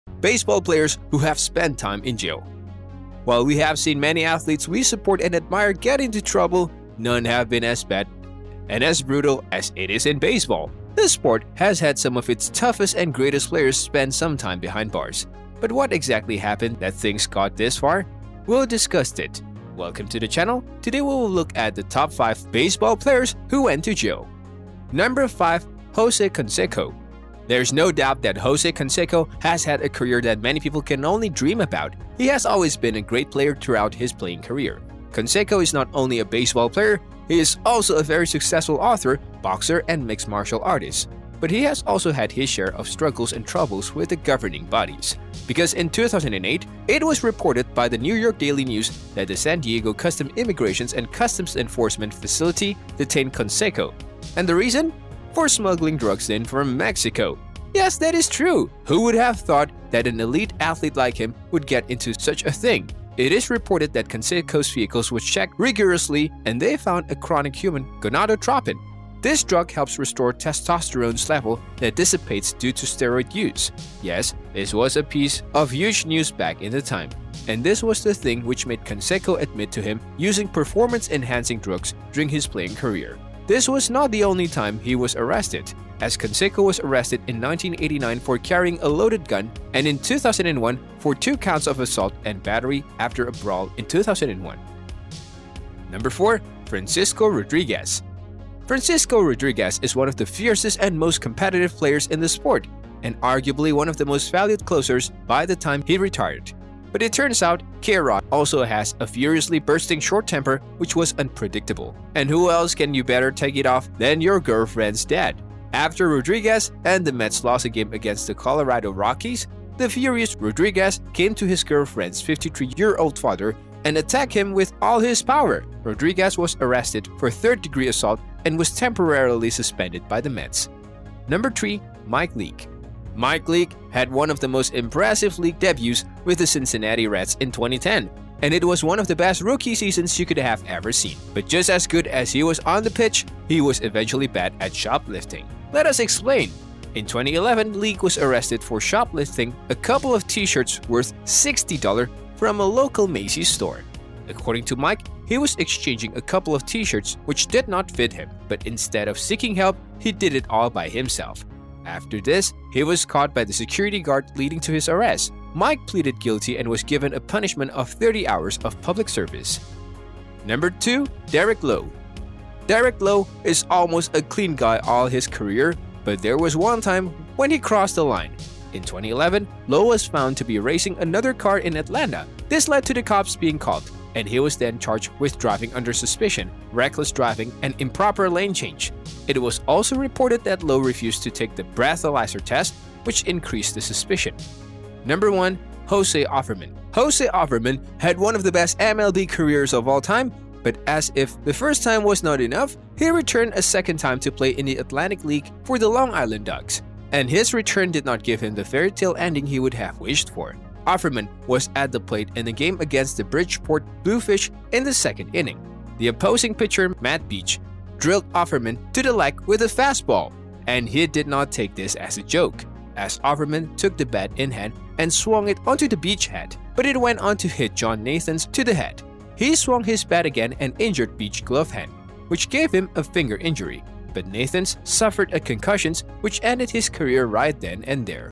Baseball Players Who Have Spent Time in Jail
In today's Baseball Busters video we are going to be looking at Baseball Players Who Have Spent Time in Jail